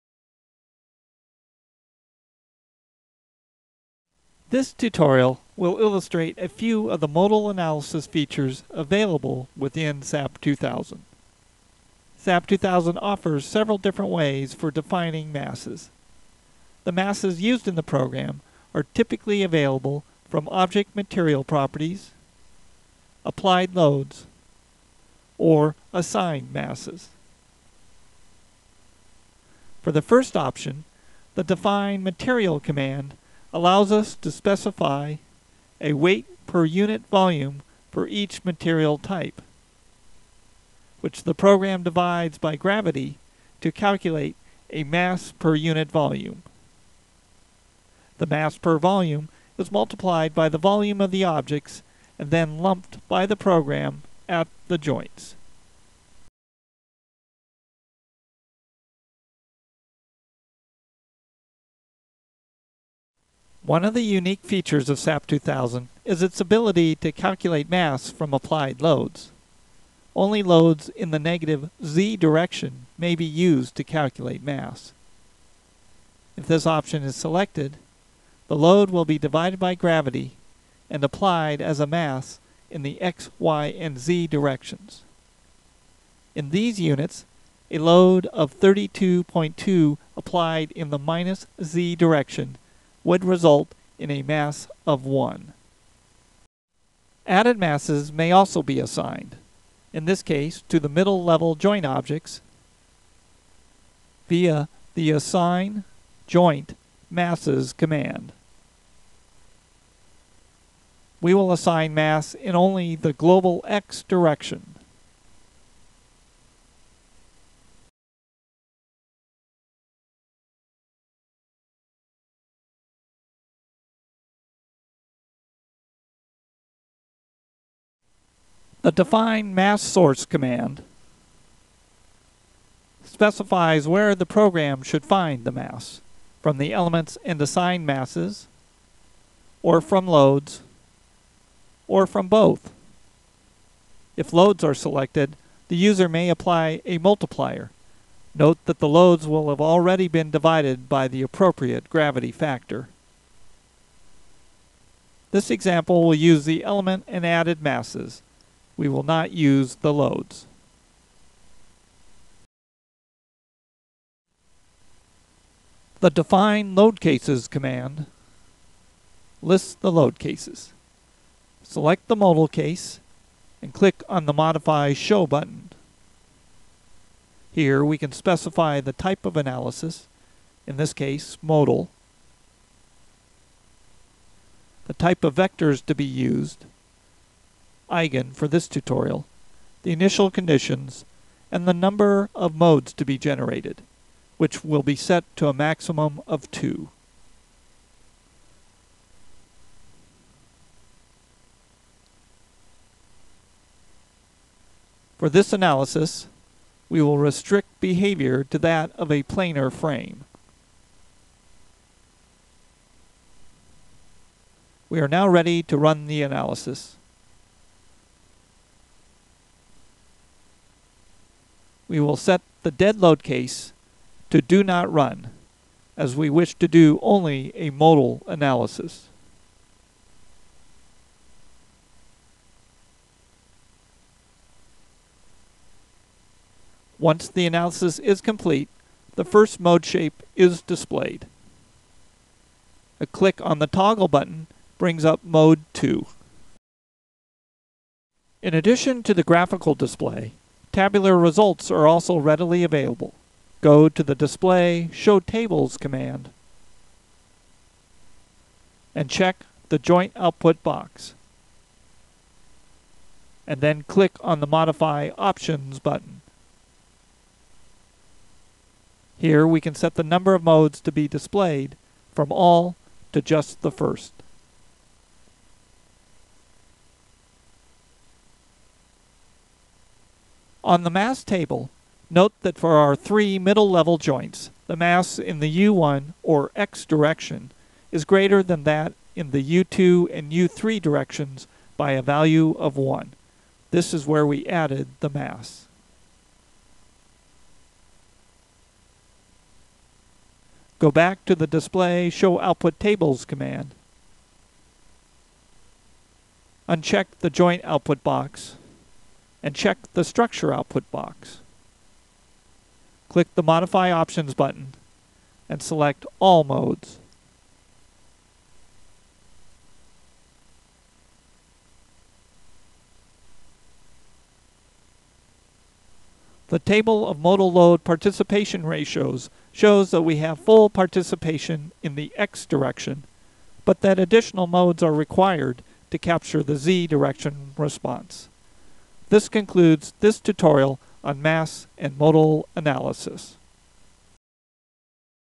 SAP2000 - 04 Mass and Modal Analysis: Watch & Learn
Learn about the SAP2000 3D finite element based structural analysis and design program and the ways that mass may be specified and the types of modes available for dynamic analyses.